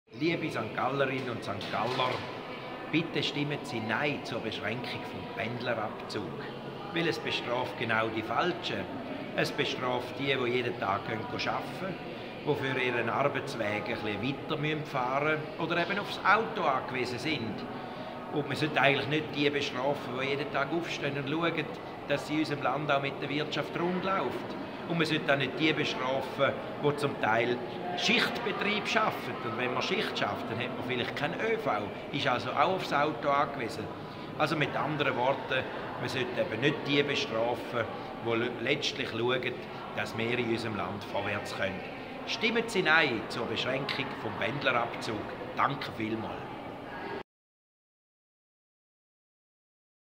Toni Brunner: Nein zur Beschränkung des Pendlerabzugs
Kanton St. Gallen
Am 15. November 2015: Nein zur Beschränkung des Pendlerabzugs

Das überparteiliche Komitee „Nein zur Beschränkung des Pendlerabzugs“ reichte am 27. April 2015 bei der Staatskanzlei mit 6745 beglaubigten Unterschriften das Referendum gegen den XI. Nachtrag zum Steuergesetz ein. Daher kommt es im Kanton St.Gallen am 15. November 2015 zu einer Volksabstimmung über die Beschränkung des Pendlerabzugs. Bei der Unterschriftensammlung erfuhr das Komitee viel Zuspruch gegen die Beschränkung des Pendlerabzugs. Die von der St.Galler Regierung und dem Kantonsrat beschlossene Beschränkung würde ab dem Jahr 2016 eine Steuererhöhung von 30 Millionen Franken zu Lasten von 60‘000 Arbeitnehmerinnen und Arbeitnehmer bedeuten, die für ihren Arbeitsweg auf das Auto oder in spezieller Weise auf den öffentlichen Verkehr angewiesen sind.